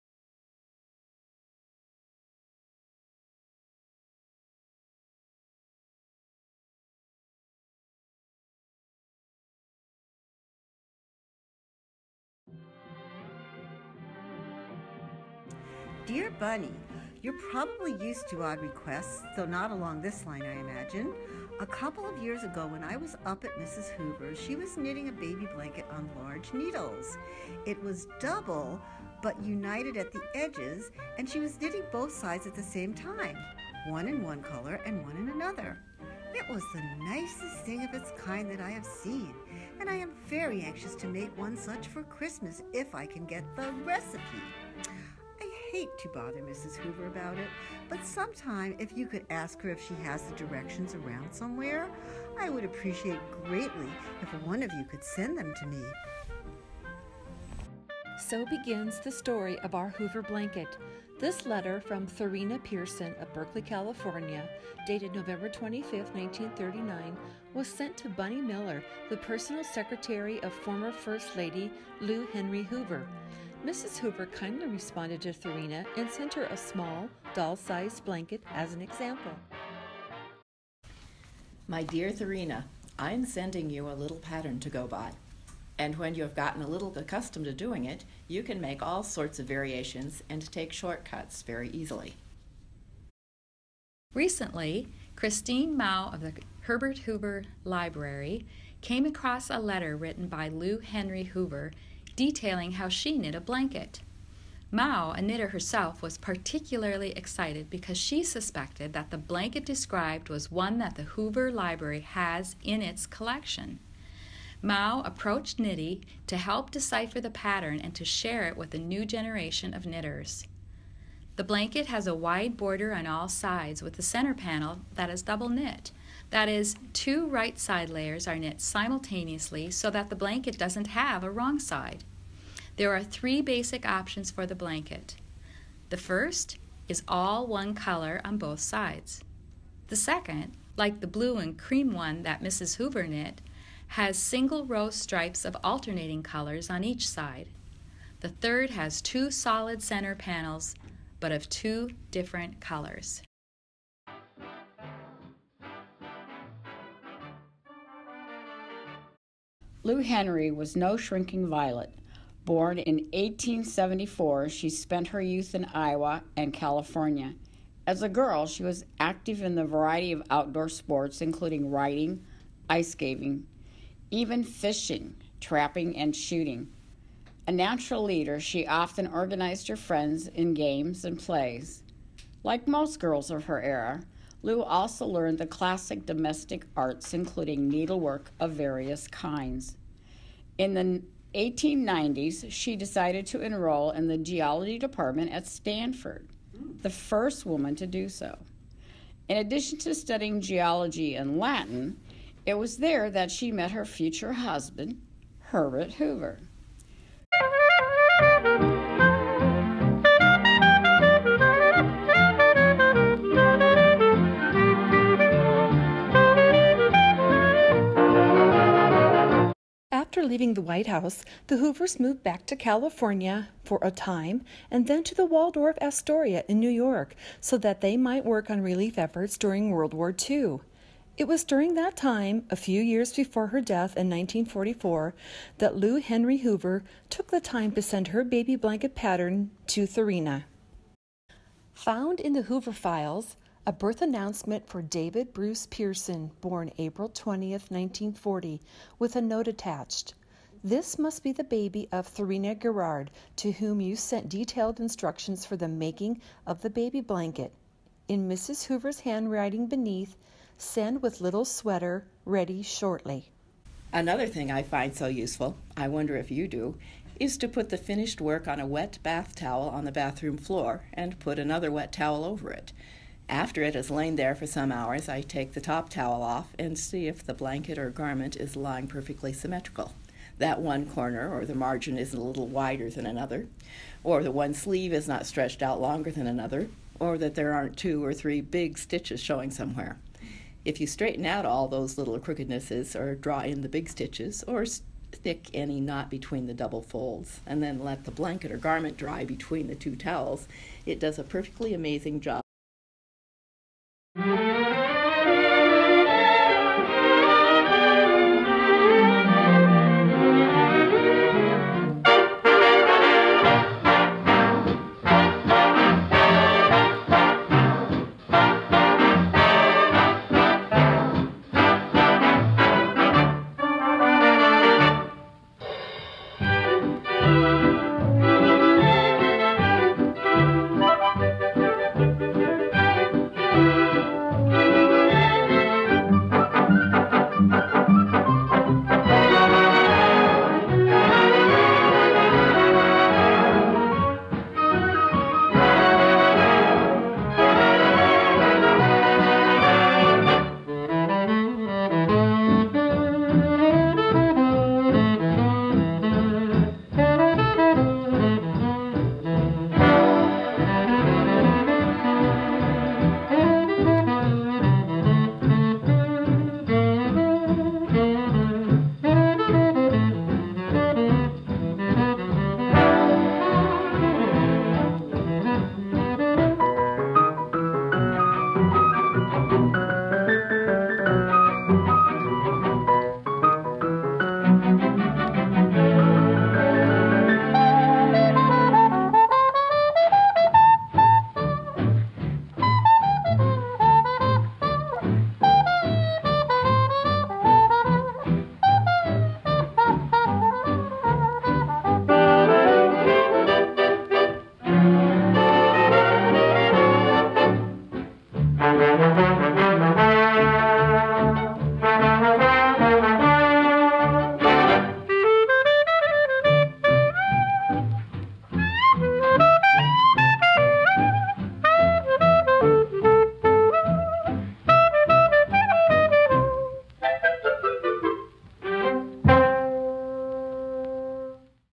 The Hoover Blanket
This video is about The Hoover Blanket. Published in Knitty. by designer, Kristi Porter, some excerpts from her pattern are used in this video. Pictures are from Google Images and knitted Hoover Blankets are from Ravelry.com. Thank you knitters and members from Prairie Yarn Over for voice overs!

Of note, one of the top tunes in 1940 is by master clarinetist, Artie Shaw and is nothing but frosting.